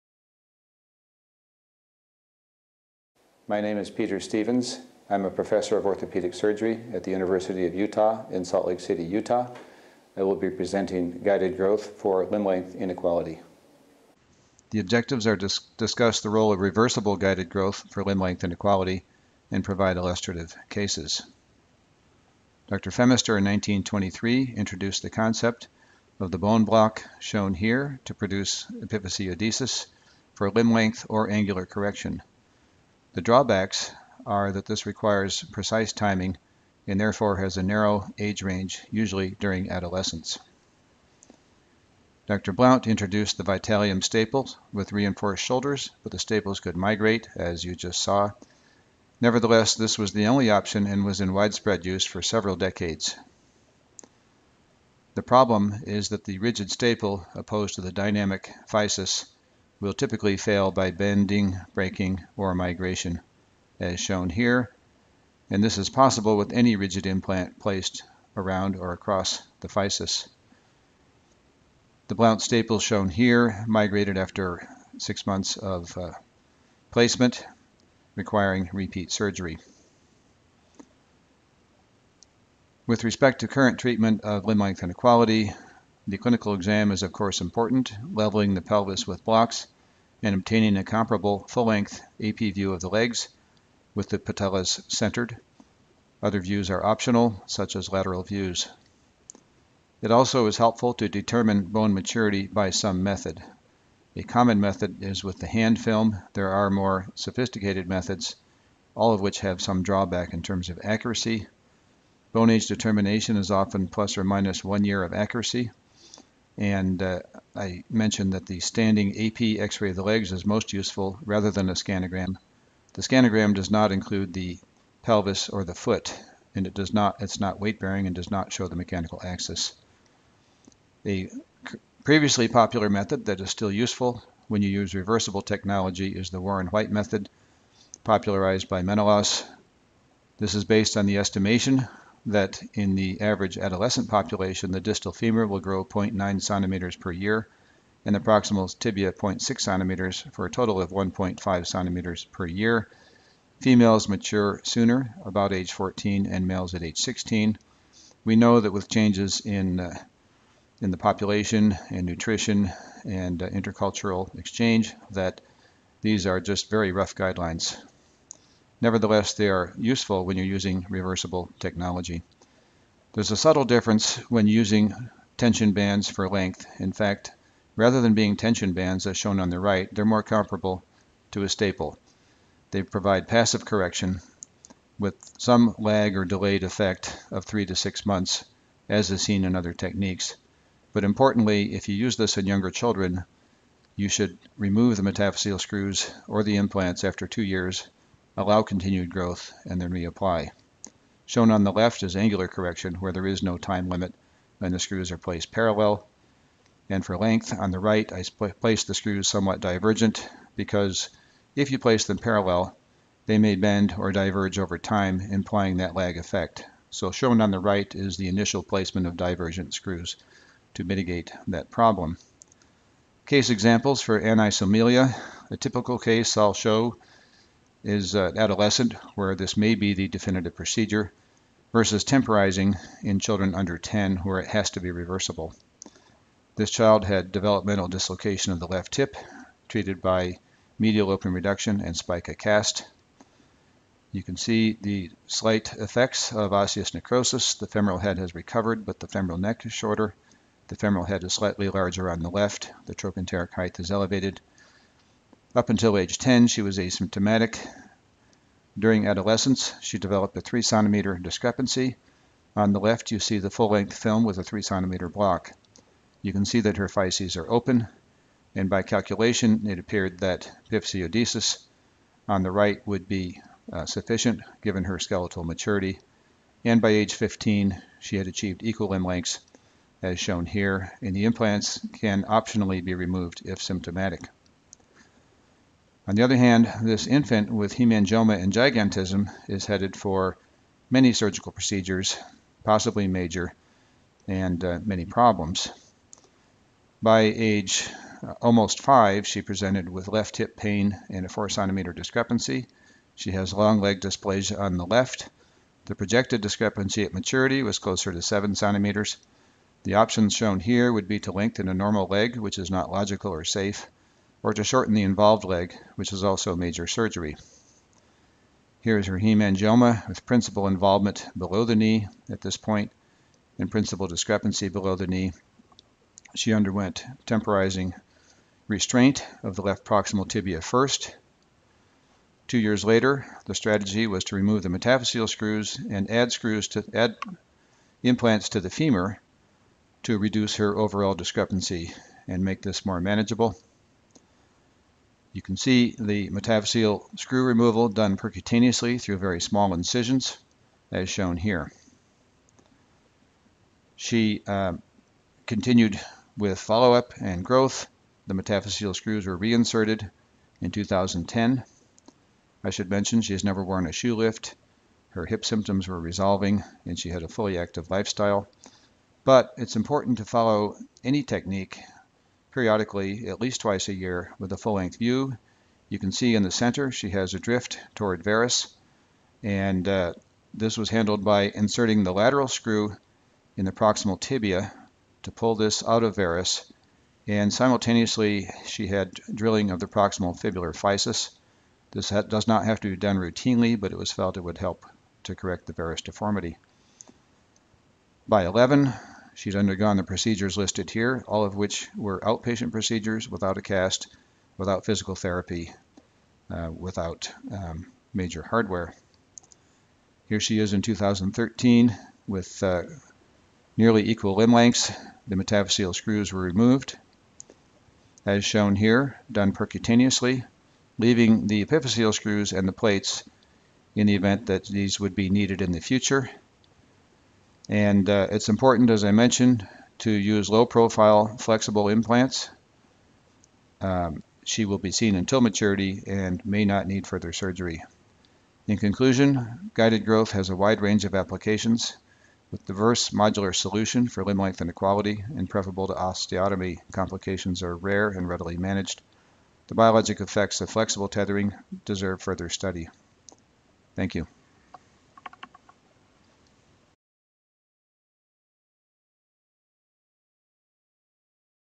Guided Growth: Limb-Length Inequality
Peter M. Stevens, MD discusses guided growth for limb-length inequality.